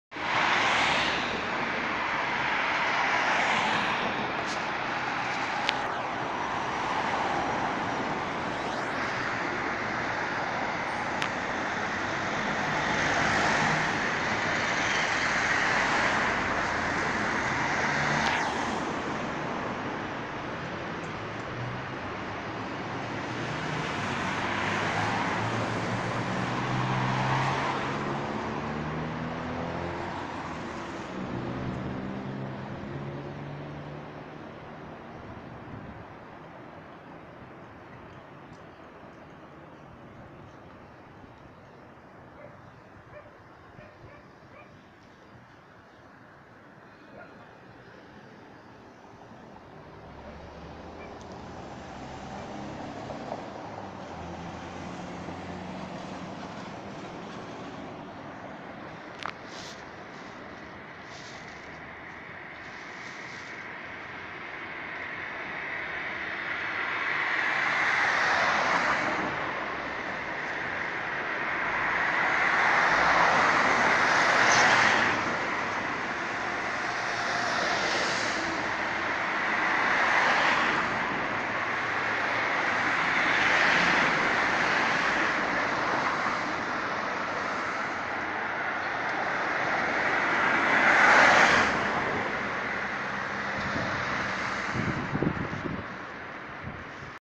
Avenida Santa Raquel, La Florida, En Horario Punta
Pasan automóviles, buses y otros vehículos, en horario punta.